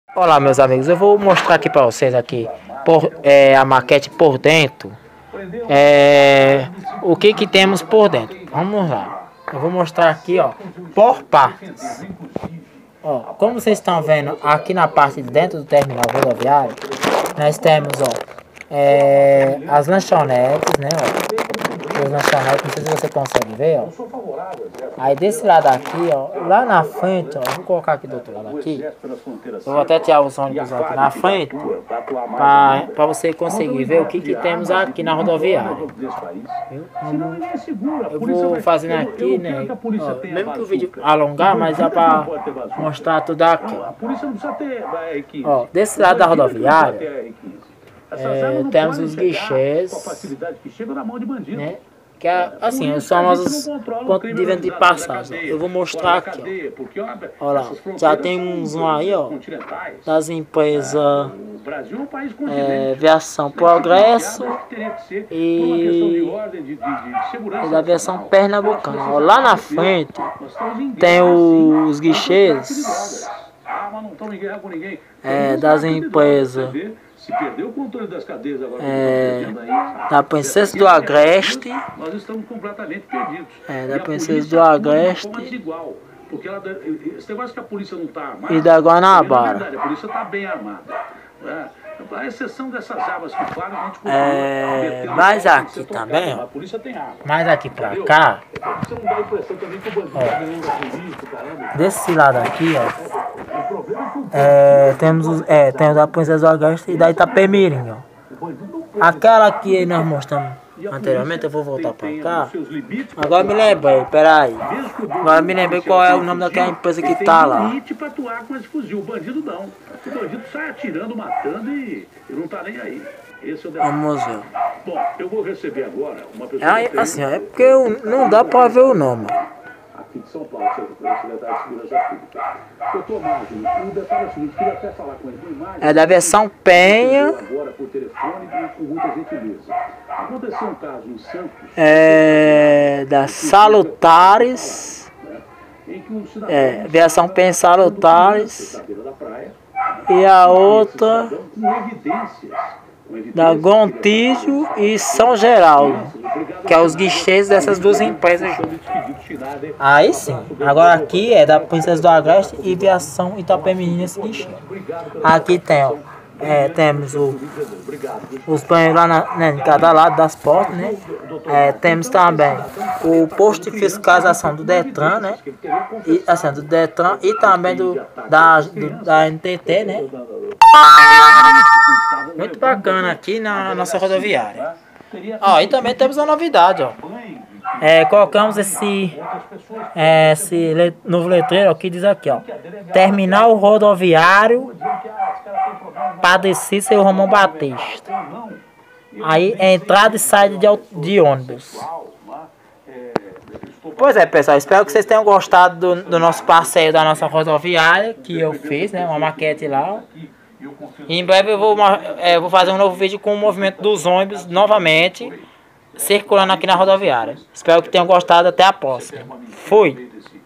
Mostrando a maquete do terminal rodoviário com detalhes
Veja como funciona este terminal rodoviário e também os guichês de venda de passagens neste lugar